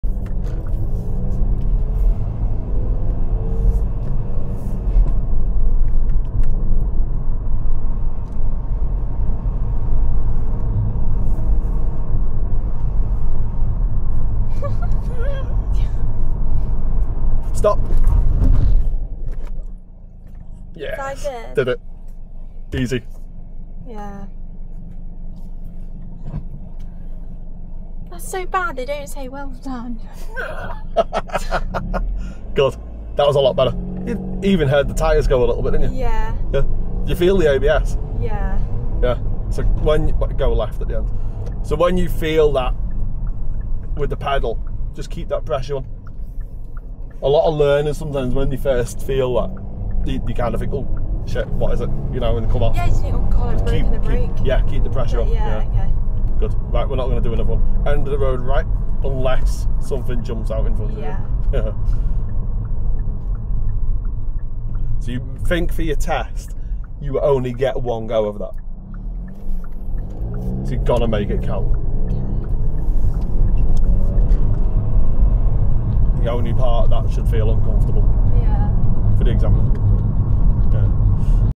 Emergency Stop Practice ✋
Poppy has been practicing the emergency stop. On this go she does really well 🙌🏼